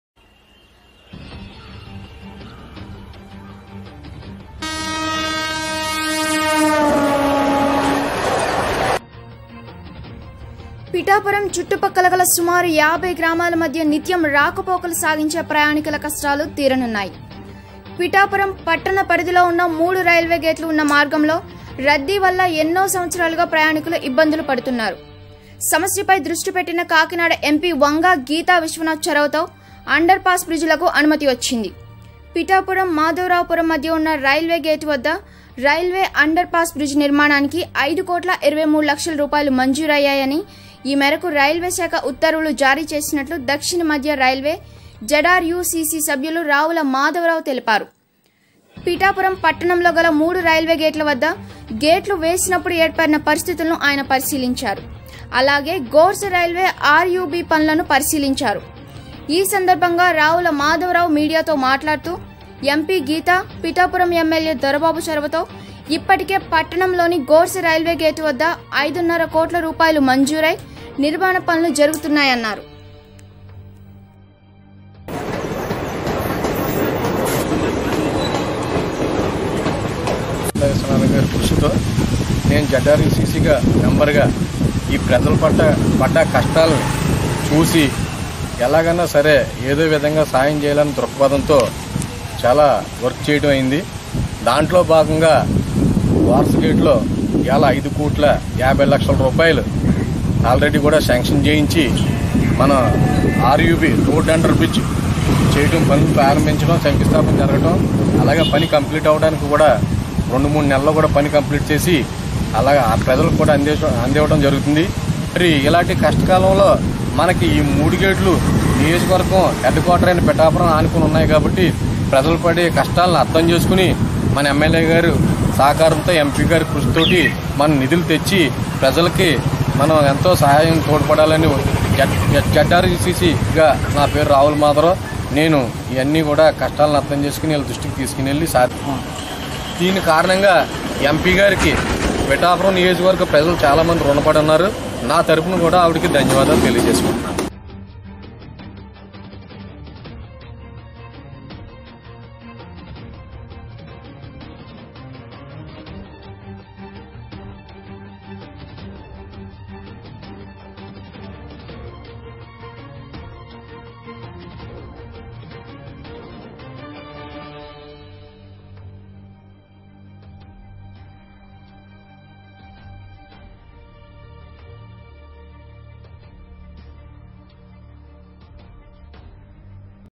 Kakinada MP Vanga Geetha viswanath#Development Railway Under pass Bridges in Pithapuram||Aditya9News
పిఠాపురం,చుట్టుప్రక్కలగల సుమారు యాభై గ్రామాలమధ్య నిత్యం రాకపోకలు సాగించే ప్రయాణికుల కష్టాలు తీరనున్నాయి. పిఠాపురం పట్టణ పరిధిలో ఉన్న మూడు రైల్వేగేట్ లు ఉన్న మార్గంలో రద్దీ వల్ల      ఎన్నో సంవత్సరాలుగా ప్రయాణికులు ఇబ్బందులు పడుతున్నారు. సమస్య పై దృష్టి పెట్టిన కాకినాడ ఎంపీ వంగా గీతావిశ్వనాథ్  చొరవతో అండర్ పాస్ బ్రిడ్జి లకు అనుమతి వచ్చింది. పిఠాపురం-మాధవపురం మధ్య ఉన్న రైల్వేగేటు వద్ద రైల్వే అండర్ పాస్ బ్రిడ్జి నిర్మాణానికి 5 కోట్ల 23 లక్షలరూపాయలు మంజూరయ్యాయని,ఈ మేరకు రైల్వేశాఖ ఉత్తర్వులు జారీచేసినట్లు దక్షిణమధ్య రైల్వే జడ్.ఆర్.యూ.సీ.సీ.సభ్యులు రావుల మాధవరావు తెలిపారు.పిఠాపురం పట్టణంలోగల మూడు రైల్వే గేట్లవద్ద గేట్లు వేసినప్పడు ఏర్పడిన పరిస్థితులను ఆయన పరిశీలించారు. అలాగే గోర్స రైల్వే ఆర్.యూ.బీ.పనులను పరిశీలించారు.ఈ సందర్భంగా రావుల మాధవరావు మీడియాతో మాట్లాడుతూ. ఎంపీ గీత,పిఠాపురం ఎమ్మెల్యే దొరబాబు చొరవతో ఇప్పటికే పట్టణంలోని గోర్స రైల్వేగేటువద్ద అయిదున్నర కోట్లరూపాయలు మంజూరై,నిర్మాణ పనులు జరుగుతున్నాయన్నారు.Aditya9News